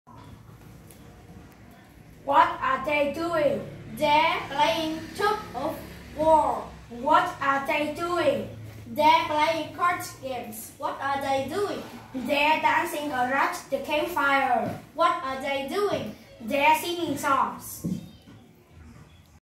#37. What are they doing?
- Briefly asking and answering about what others are doing at the campsite.
+ Q: Why are they doing?
+ A: They're playing tug of war/ playing card games/ dancing around the campfire/ sing songs.